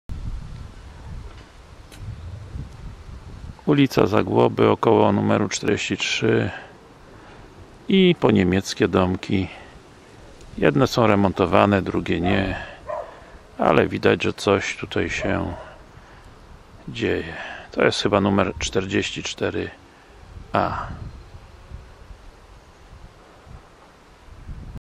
Wrocław Żerniki ulica Zagłoby
Wrocław Żerniki ulica Zagłoby na skrzyżowaniu z Berberysową. Widać przedwojenny poniemiecki domek, który obecnie jest w trakcie odświeżania elewacji. Te domki są nieco mniejsze niż na ulicy Komeńskiego i prawdopodobnie wybudowane nieco wcześniej.